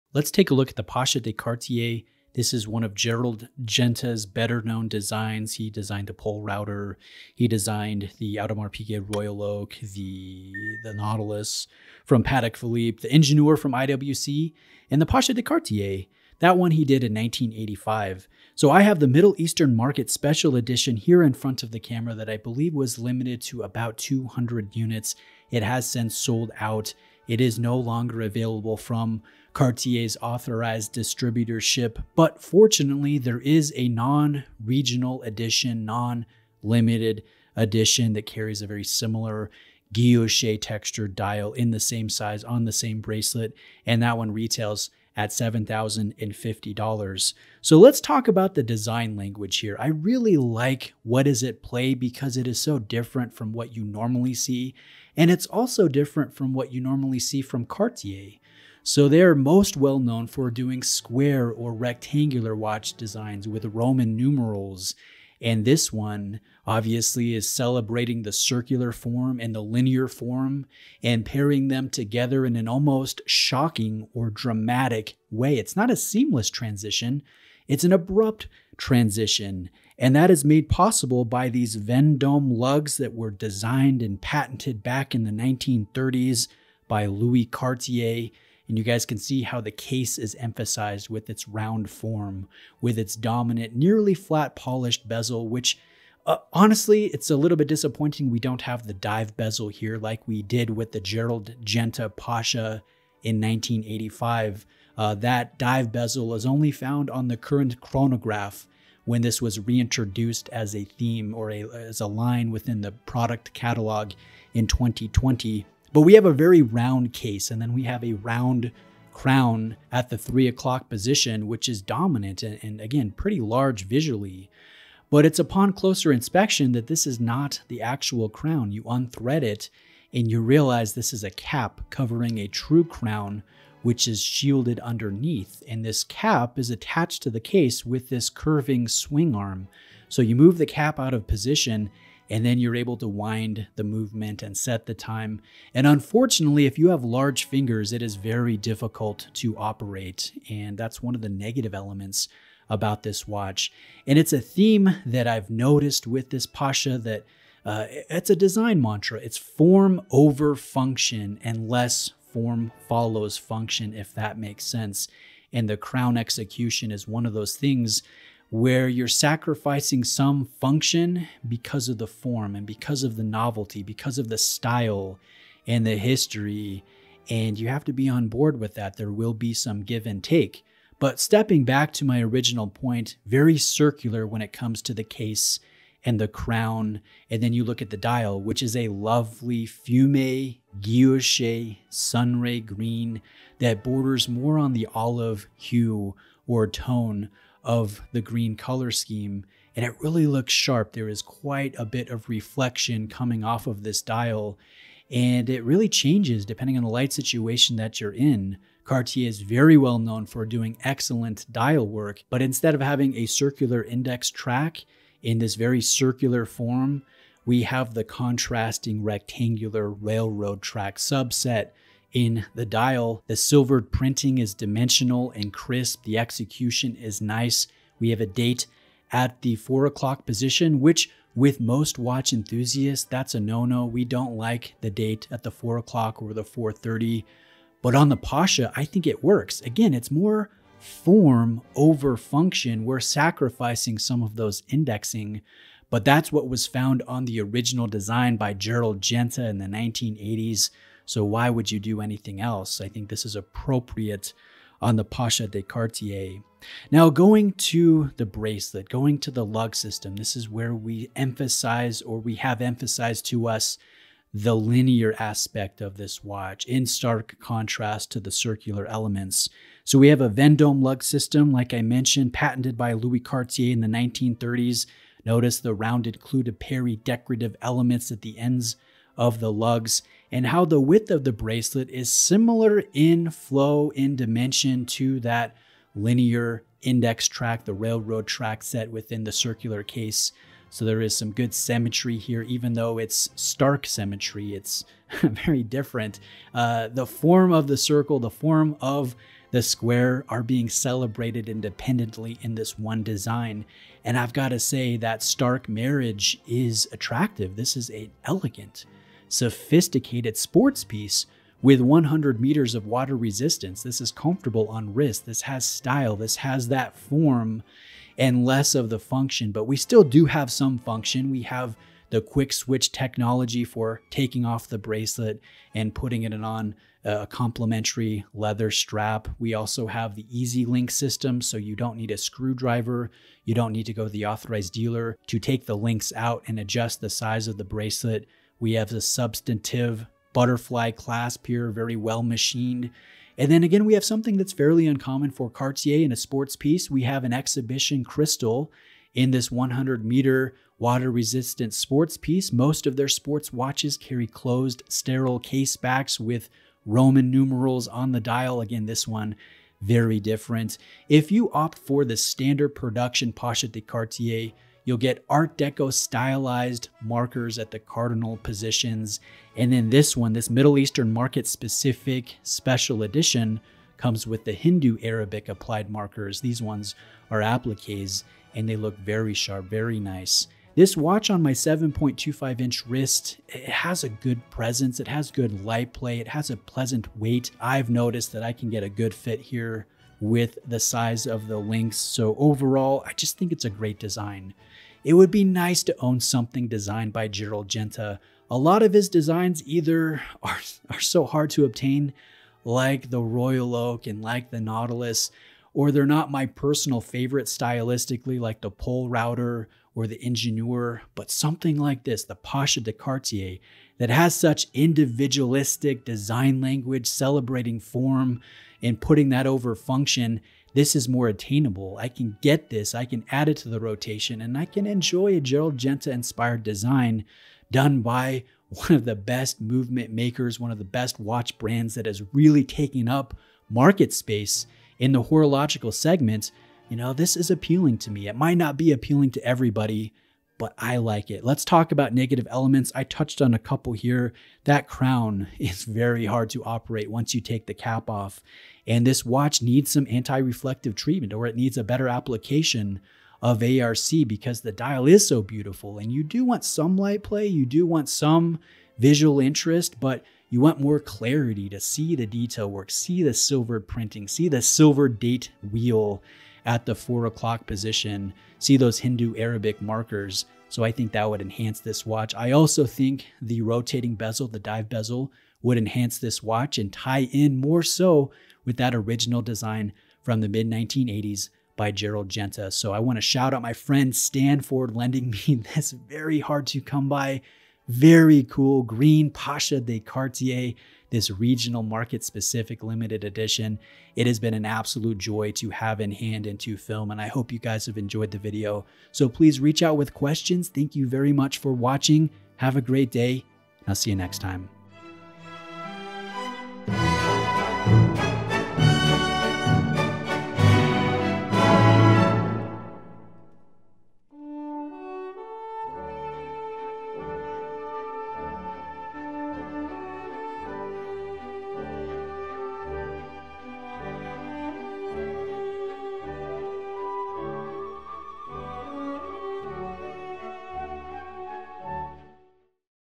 The Attainable Gerald Genta Design - Pasha De Cartier
One of Gerald Genta's better known watch designs- the Pasha de Cartier has a lovely individualistic style. It is different from other Cartier sport models. Yet it doesn't look out of place in the product line up. This limited version meant for the Middle Eastern market is especially sharp. If you are sleeping on this brand, you are missing out.

A kind heartfelt thank you to Stan for letting me spend time with and film is rare watch!